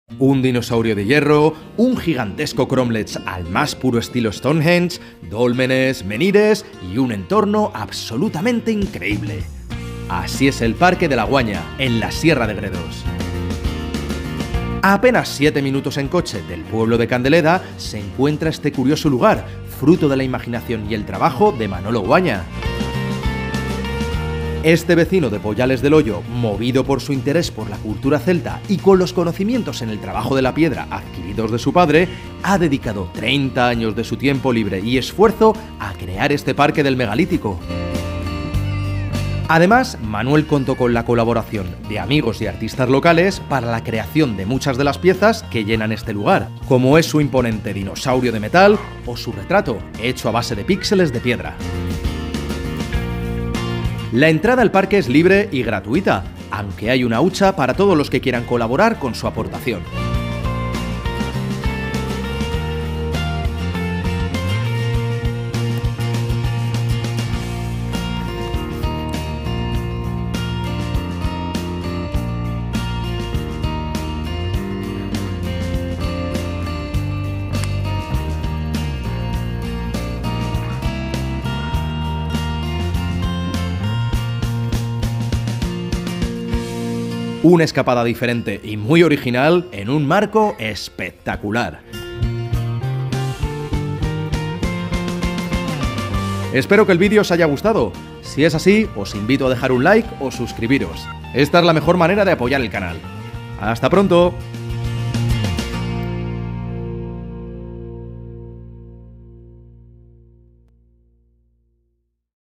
España Oculta: PARQUE DEL MEGALÍTICO "LA GÜAÑA"
En ocasiones la locura de alía con el ingenio para dar lugar a sitios increíbles que sólo pueden salir de la imaginación. Este es el caso del Parque de la Güaña, en la Sierra de Gredos, un espacio increíble dedicado al megalítico en un entorno absolutamente maravilloso junto al pueblo de Candeleda.